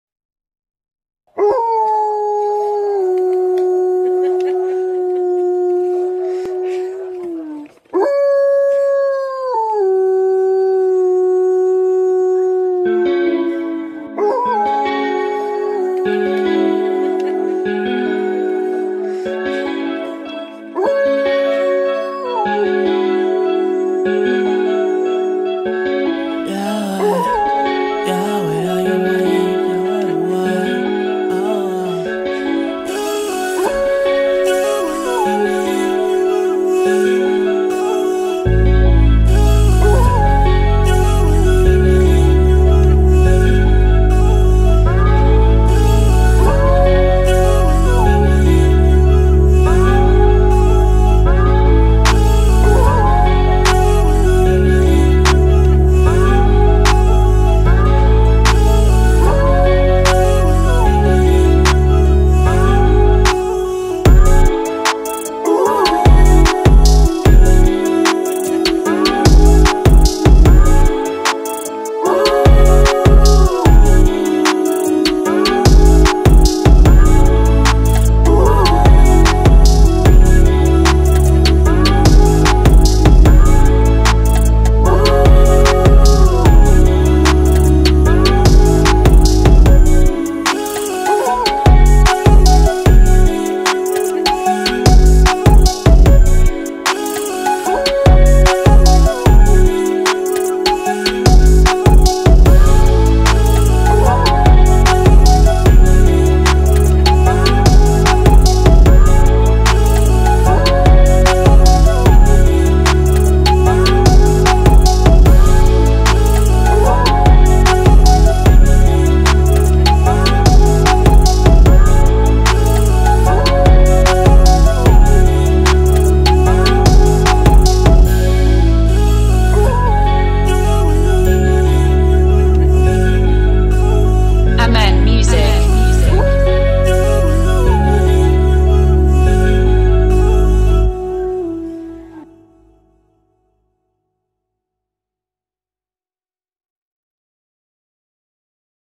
Comment faire un beat avec un Chien malamute (ooouuuuuhhh)
How to make a beat with a malamute dog
comment collaborer avec un chien malamute qui hurle ? 🐕‍🦺